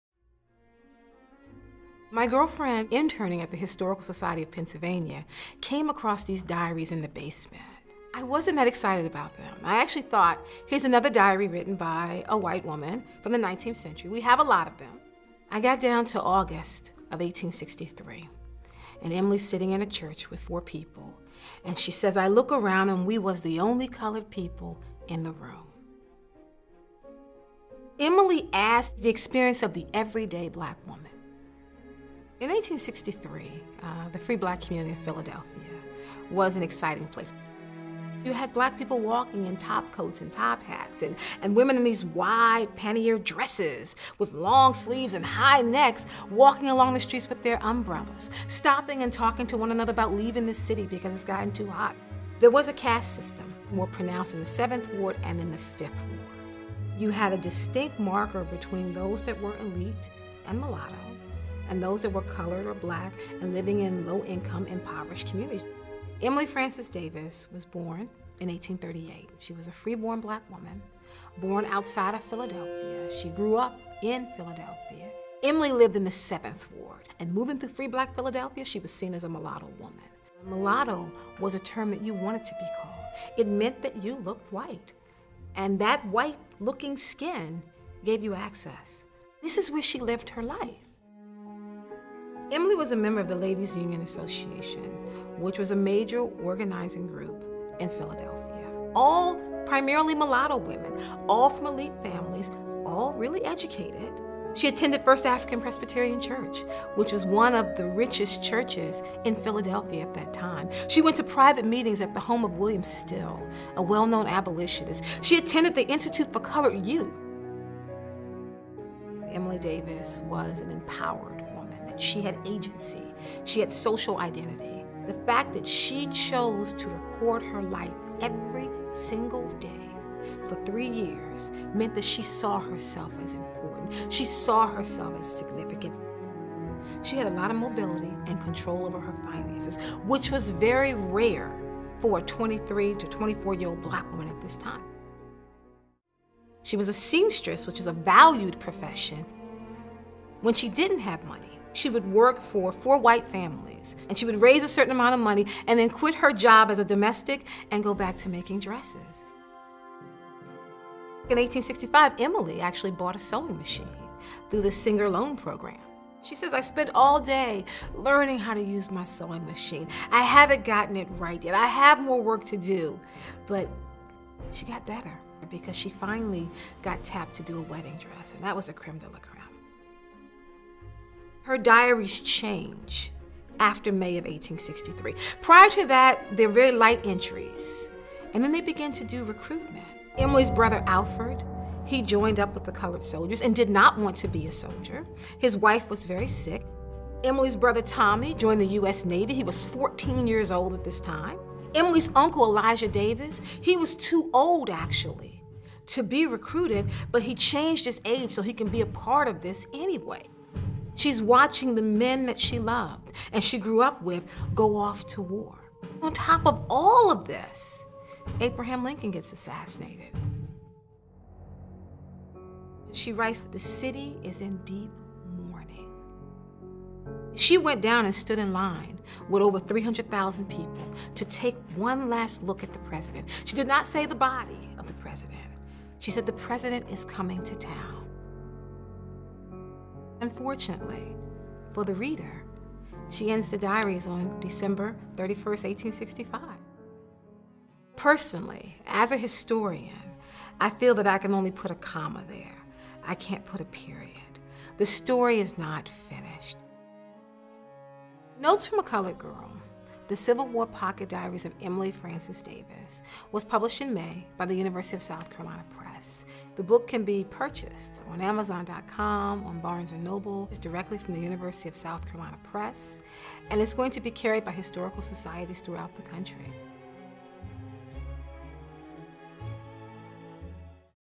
Notes From A Colored Girl: The Civil War Pocket Diaries of Emilie Frances Davis
Karsonya "Kaye" Wise Whitehead takes us through the life of a middle class seamstress who wrote one of the only preserved diaries of a black woman in the country.

Like what you see? Keep up with the Women of Philadelphia project!